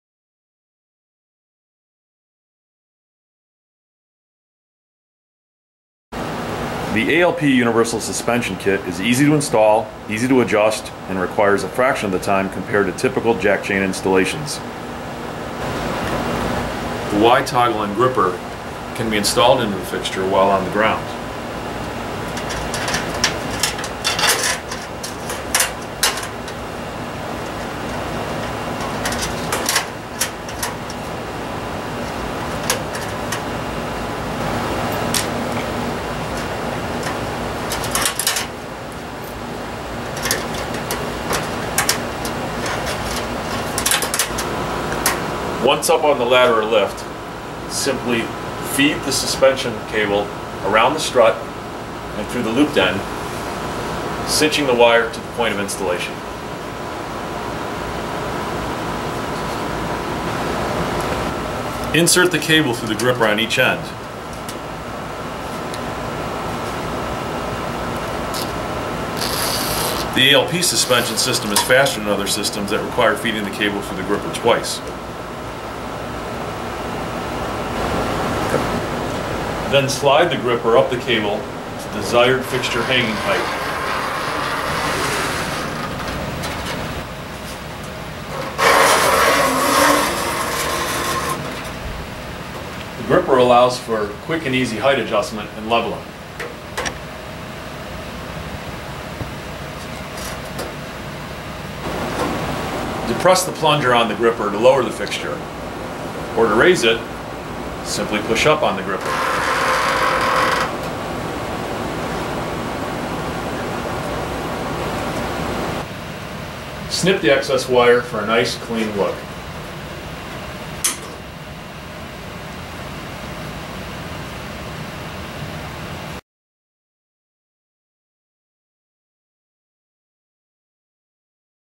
A.L.P. Universal Suspension System
Universal Yoke and Toggle Suspension Kit

Lighting grippers and suspension kits from A.L.P. offer value, versatility and field adjust-ability. Featuring the latest technology in ceiling fixture suspension, A.L.P.s systems are both durable and flexible. Fixtures are raised and lowered on the suspension wire for fast, cost-effective installation and easy adjustment, with significantly lower labor cost. Crafted of the highest quality material, including internal stainless steel ball bearings, this kit offers the integrity that lighting OEMs require. Available as a complete kit with two (2) gripper assemblies and cable sets per kit, in order to hang a complete fixture with one kit.